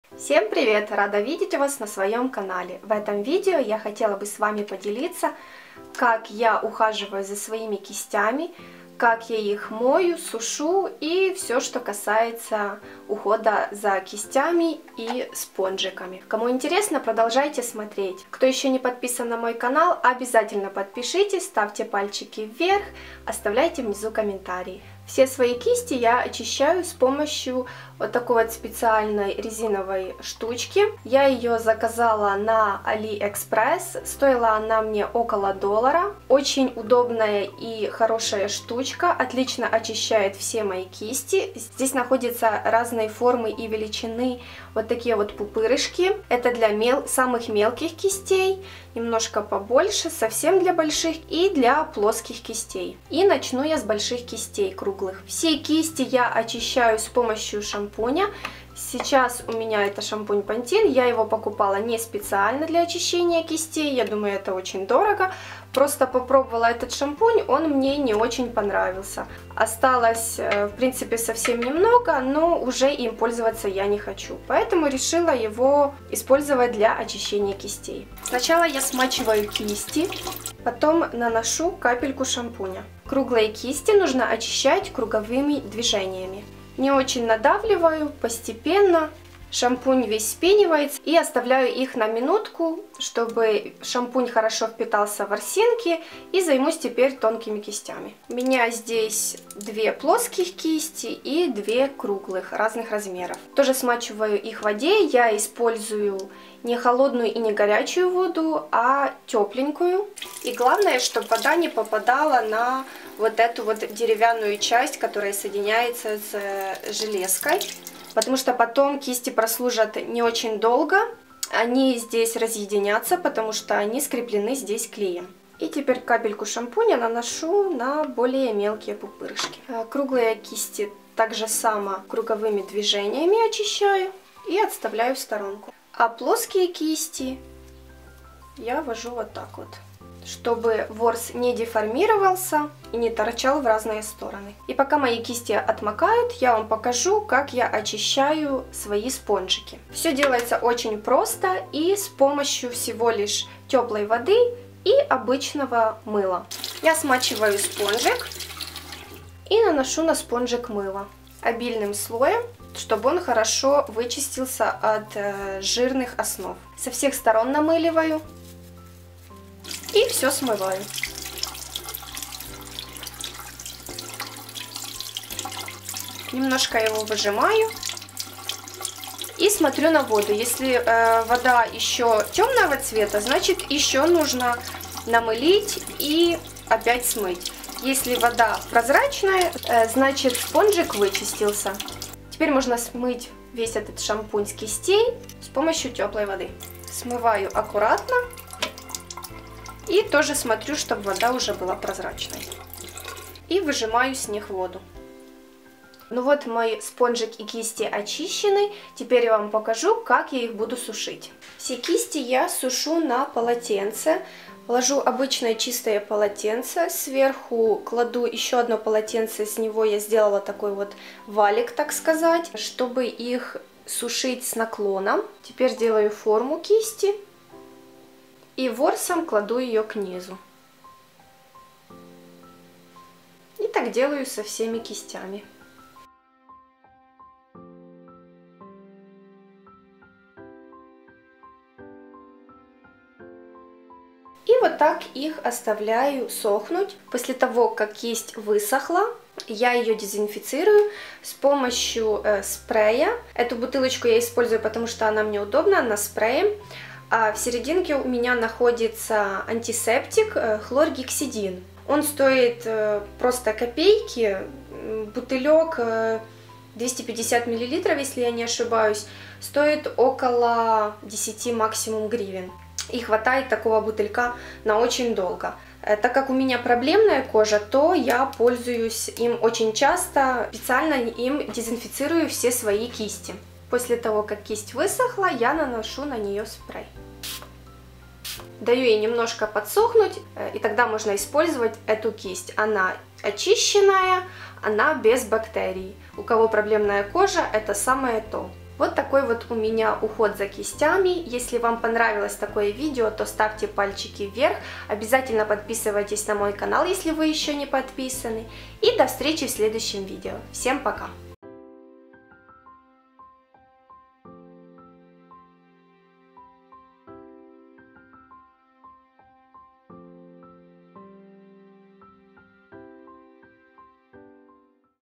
Как ухаживать за кистями/LJ
Хорошие кисти для макияжа могут служить долго. Главное, уделять им время и стараться обеспечить хороший уход.
В этом видео я покажу вам как я это делаю.
Ничего сложного в уходе за кистями нет, главное делать это 1 раз в неделю и тогда ваша кожа скажет вам спасибо и на ней не будут появляться новые прыщички и воспаления!)
Хорошие кисти для макияжа можно считать одним из главных атрибутов красивого мейка, ведь во многом именно от них зависит, как косметика будет ложиться на лицо. Поскольку данные инструменты постоянно контактируют с кожей, требования к их чистоте, естественно, большие. Неправильный уход может, как испортить сами кисти, так и добавить проблем с лицом: прыщи, забитые поры и так далее.
Так что смотрите мой вариант очищения и сушки, и обязательно поделитесь внизу как вы ухаживаете за кистями, и может у вас есть какие то свои секреты(лайфхаки) в уходе за кистями и спонжиками)))
Вы же не хотите пропустить новое виде на канале?))))

➜Где меня искать?